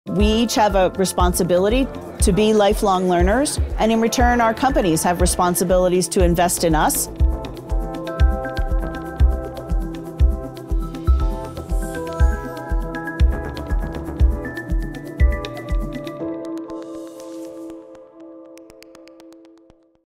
Growth Summit 2023: Kathy Wengel
Join mission-oriented leaders discussing a fairer future of work. The World Economic Forum's 'Growth Summit: Jobs and Opportunity for All' kicks off on 2 May.